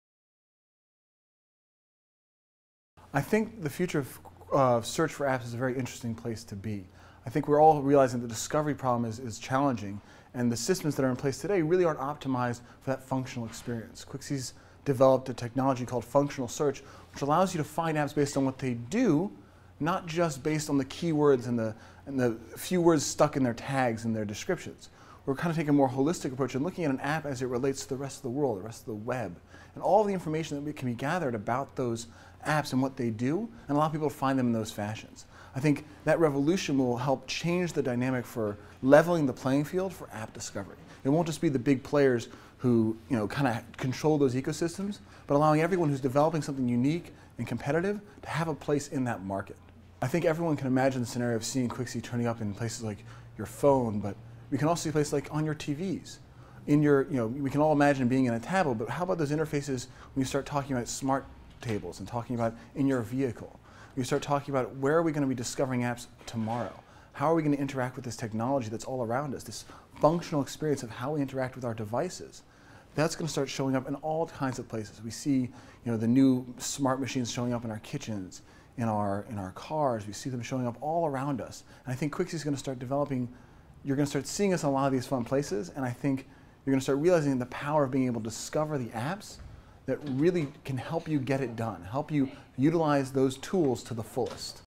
Jacob Orrin, Quixey - appbackr's Funding, Financing, and Distributing Apps Conference
An interview with Jacob Orrin, Director of Business Development at Quixey, at the Funding, Financing, and Distributing Apps Conference. The conference was hosted by appbackr and held on May 31, 2012 in Palo Alto, CA.

To view all the video interviews and a recap of the conference, go